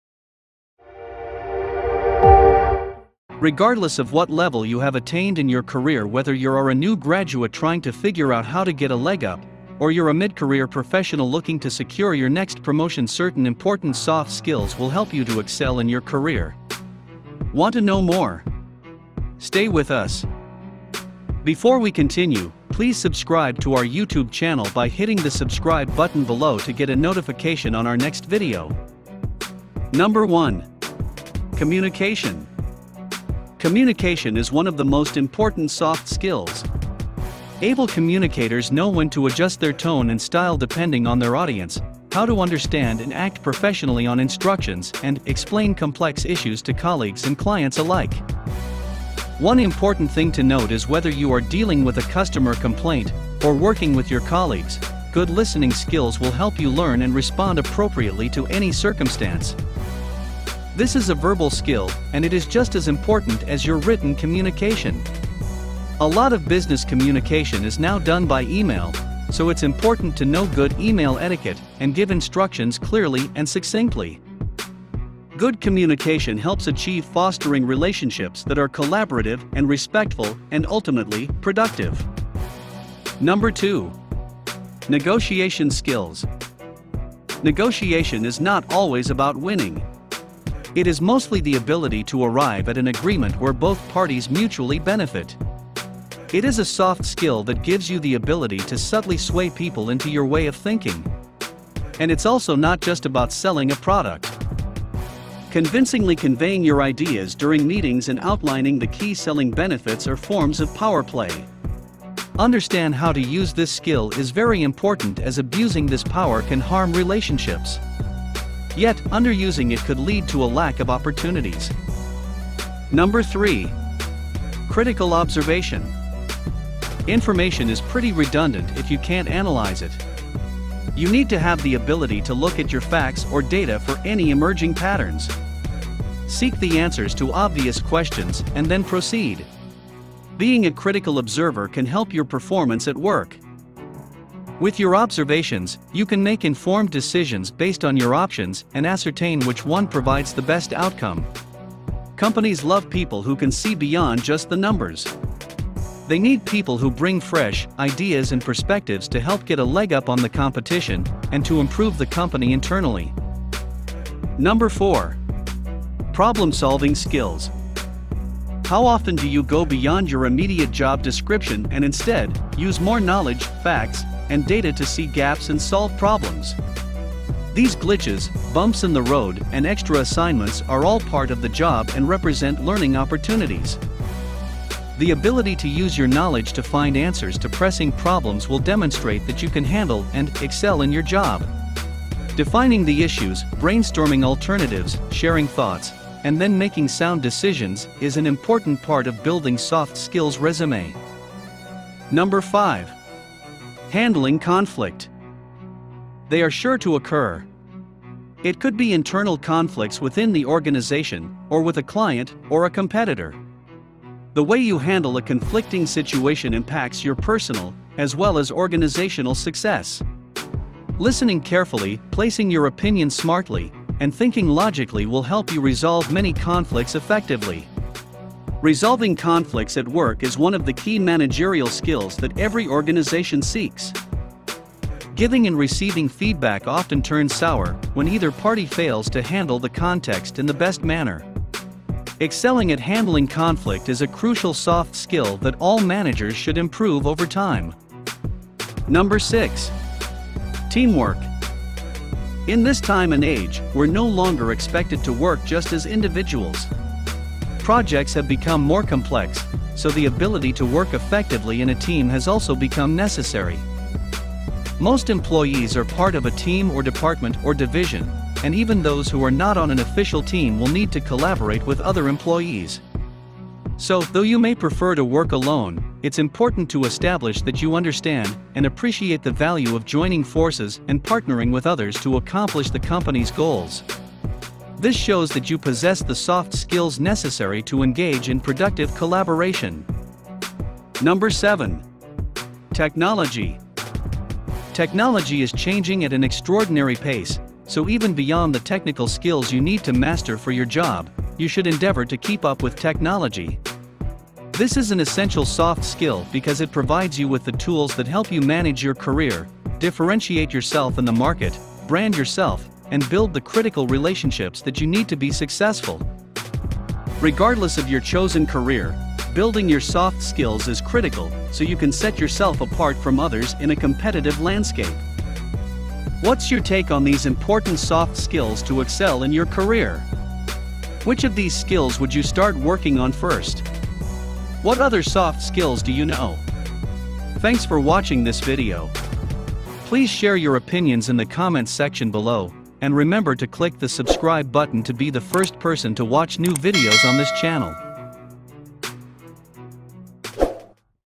Important Soft Skills to Excel in Your Career 2023
Regardless of what level you have attained in your career - whether you're are a new graduate trying to figure out how to get a leg up, or you're a mid-career professional looking to secure your next promotion - certain important soft skills will help you to excel in your career. Want to know more? Stay with us!

1. Communication.
Communication is one of the most important soft skills. Able communicators know when to adjust their tone and style depending on their audience, how to understand and act professionally on instructions and, explain complex issues to colleagues and clients alike.
One important thing to note is whether you are dealing with a customer complaint, or working with your colleagues, good listening skills will help you learn and respond appropriately to any circumstance.

2. Negotiation Skills.
Negotiation is not always about winning. It is mostly the ability to arrive at an agreement where both parties mutually benefit. It is a soft skill that gives you the ability to subtly sway people into your way of thinking. And it’s also not just about selling a product.

3) Critical Observation.
Information is pretty redundant if you can’t analyze it. You need to have the ability to look at your facts or data for any emerging patterns. Seek the answers to obvious questions and then proceed. Being a critical observer can help your performance at work.

4. Problem-Solving Skills.
How often do you go beyond your immediate job description and instead, use more knowledge, facts, and data to see gaps and solve problems? These glitches, bumps in the road, and extra assignments are all part of the job and represent learning opportunities.

5. Handling Conflict.
They are sure to occur. It could be internal conflicts within the organization, or with a client, or a competitor. The way you handle a conflicting situation impacts your personal as well as organizational success. Listening carefully, placing your opinion smartly, and thinking logically will help you resolve many conflicts effectively. Resolving conflicts at work is one of the key managerial skills that every organization seeks.

6. Teamwork.
In this time and age, we’re no longer expected to work just as individuals. Projects have become more complex, so the ability to work effectively in a team has also become necessary.

7. Technology.
Technology is changing at an extraordinary pace, so even beyond the technical skills you need to master for your job, you should endeavor to keep up with technology. This is an essential soft skill because it provides you with the tools that help you manage your career, differentiate yourself in the market, brand yourself, and build the critical relationships that you need to be successful.